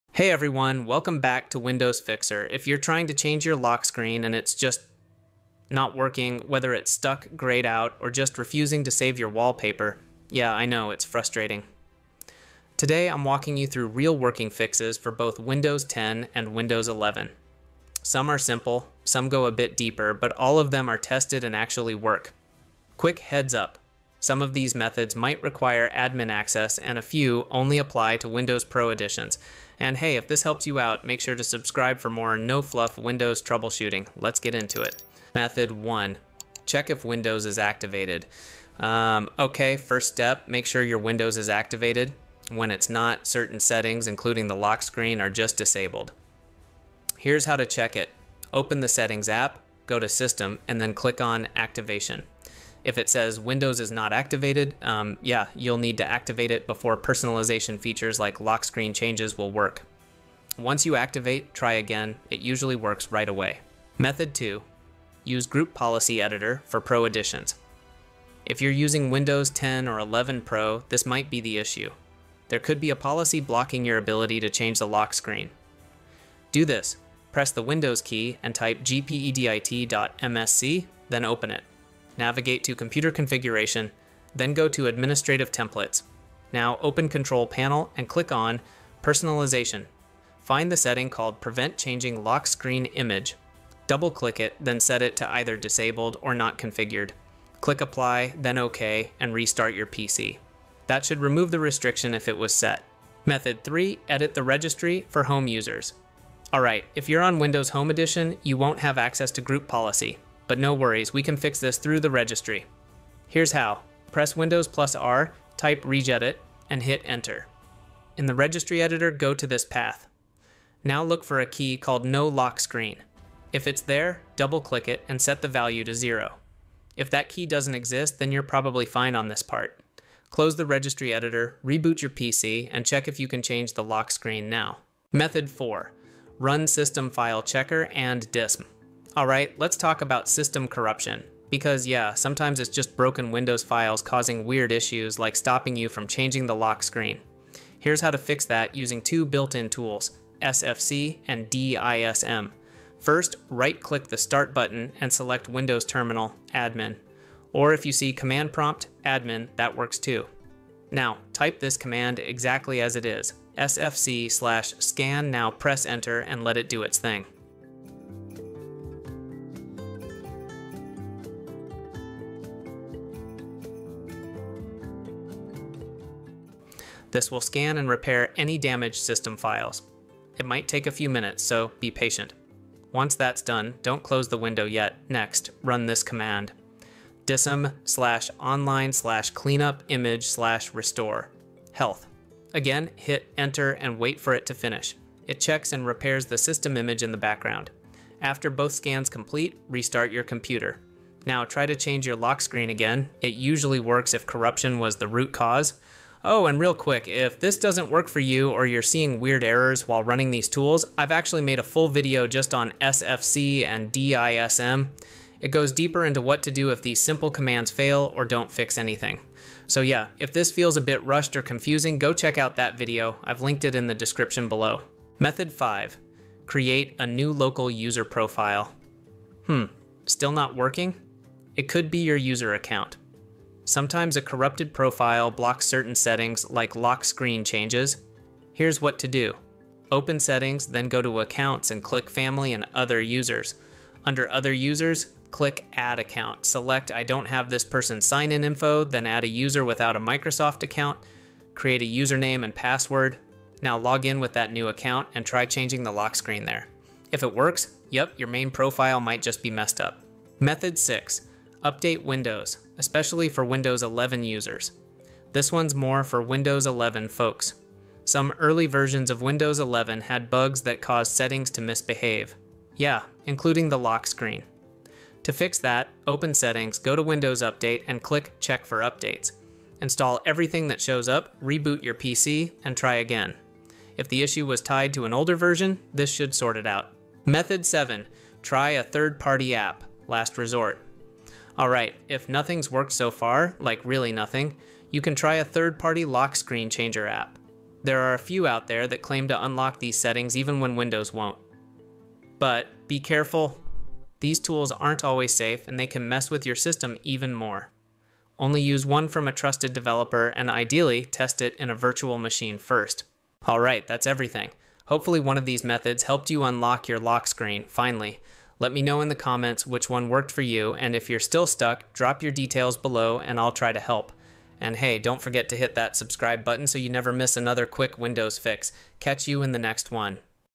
Can't Change Lock Screen in Windows 10/11? (7 Real Fixes That Work!)
Are you stuck and can’t change your lock screen wallpaper in Windows 10 or Windows 11? Whether it's greyed out, stuck on the same image, or not saving changes, this video walks you through 7 real methods to fix it.

From activation issues and group policies to registry fixes and corrupted user profiles—this video has all working solutions explained step-by-step.

Commands :
🔐sfc /scannow
🔐DISM /Online /Cleanup-Image /RestoreHealth
🛅HKEY_LOCAL_MACHINE\SOFTWARE\Policies\Microsoft\Windows\Personalization

👍 Like the video if this helped
💬 Comment your issue below